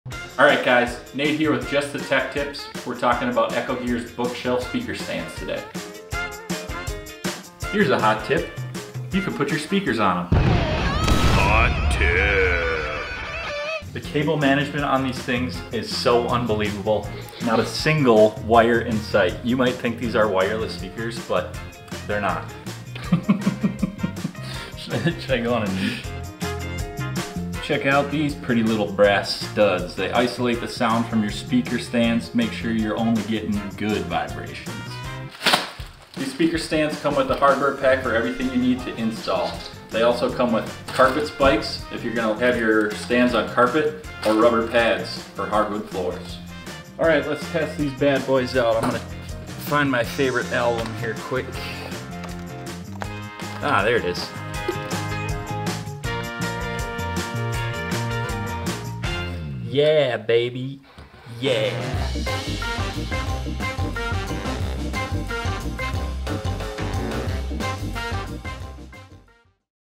The BEST Speaker Stands for Bookshelf Speakers | ECHOGEAR Speaker Stands
Elevate Your Sound Game with Echogear's Premium Bookshelf Speaker Stands!

Looking to take your home audio setup to the next level? Look no further! In this video, we're diving deep into the features of Echogear's Premium Bookshelf Speaker Stands. Whether you're a music enthusiast, podcast aficionado, or surround sound buff, these stands are designed to deliver optimal performance and stability for your prized speakers.

Here's what makes these stands stand out:

Quick and Easy Assembly: Forget about complicated setups. With all the hardware included and a simple assembly process, these stands can be ready to rock in 15 minutes or less.

Stability You Can Count On: Featuring a solid MDF post design and enlarged 6" x 6" speaker plates with rubber, vibration-reducing pads, these stands provide a stable surface for your large, premium bookshelf speakers weighing up to 25 lbs. And for extra stability, we've got you covered with museum wax or 3M Command Strips.

Versatile Flooring Options: Carpet spikes or plastic feet? Why choose when you can have both! These stands are equipped to stand tall and sturdy on any type of floor, while the vibration-reducing design ensures a buzz-free listening experience.

Cable Management Made Easy: Say goodbye to tangled cables! The oversized cable management channel accommodates cables of all sizes, from traditional speaker wires to banana plugs, keeping your setup neat and organized.

Optimal Listening Height: Standing at 28" tall, these stands elevate your speakers to the perfect height for immersive audio experiences. Whether you're enjoying music, podcasts, or surround sound, rest assured that your speakers will be where it matters most.

Ready to take your sound game to new heights?